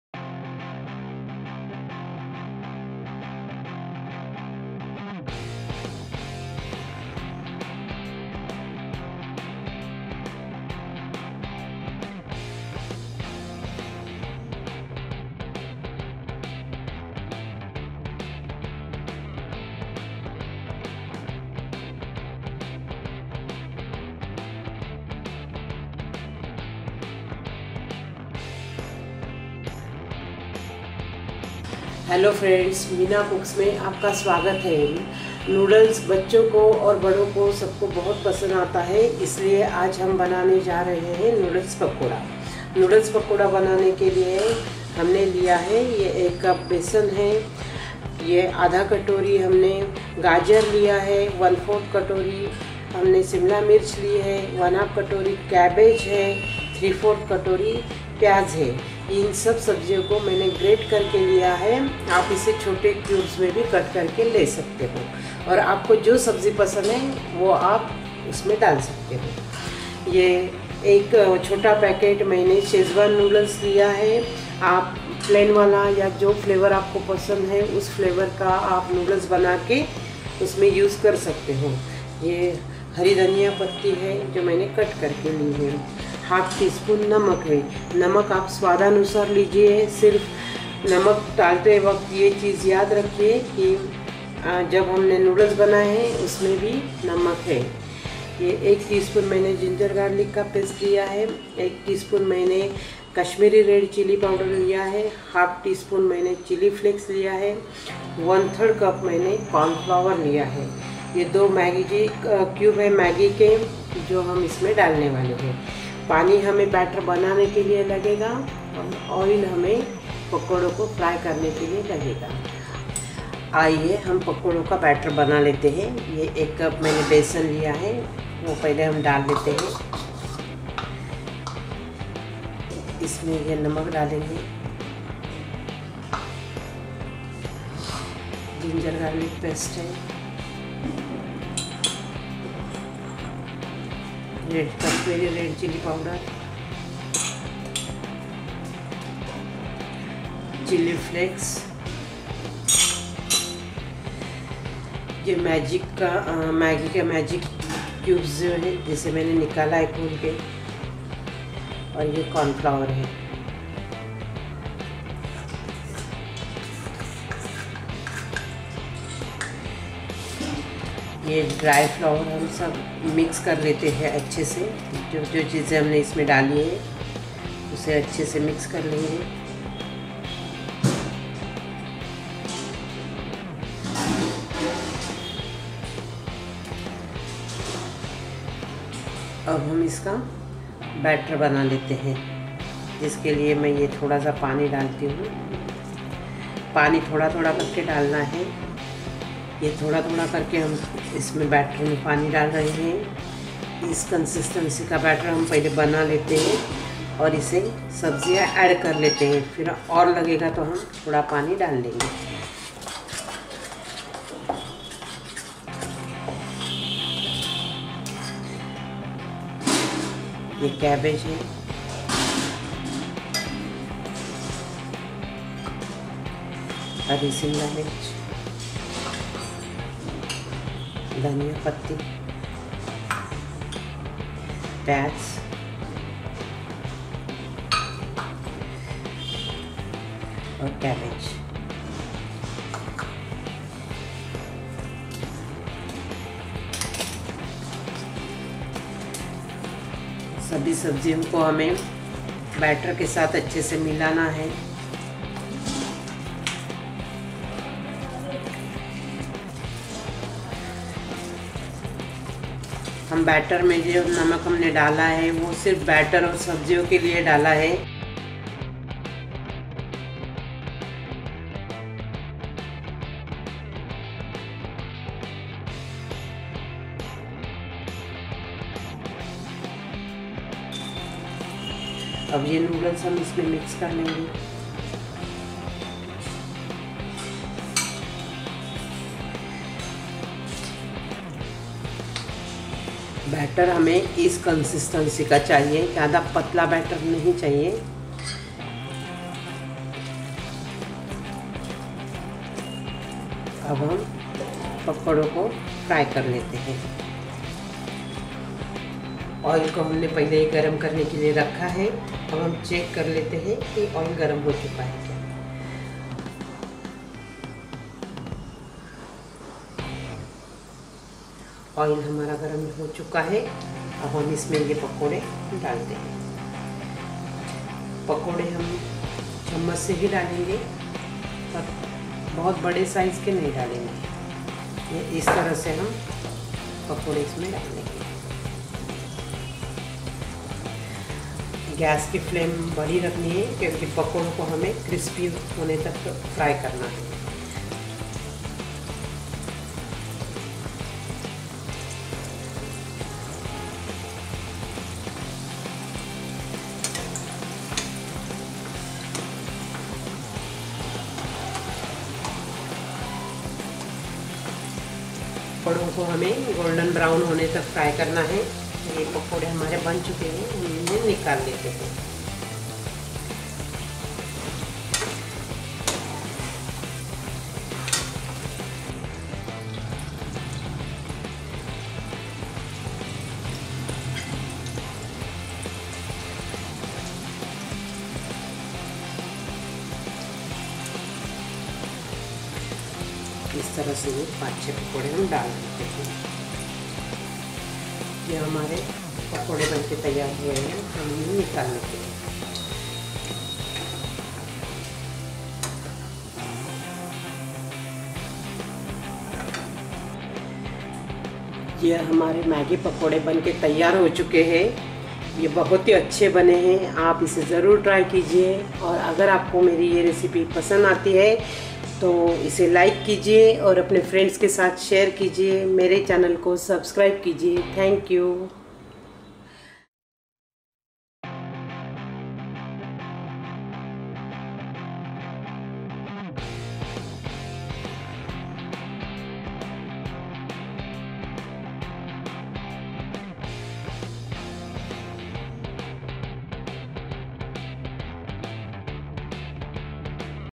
Crispy Maggi Noodle Pakoda क्रिस्पी Maggi नूडल पकोड़ा
Crispy Maggi Noodle Pakoda  is a very easy & tasty recipe by Meena Cooks. These pakodas can be served with mayonnaise or sweet chilli sauce or schezwan sauce.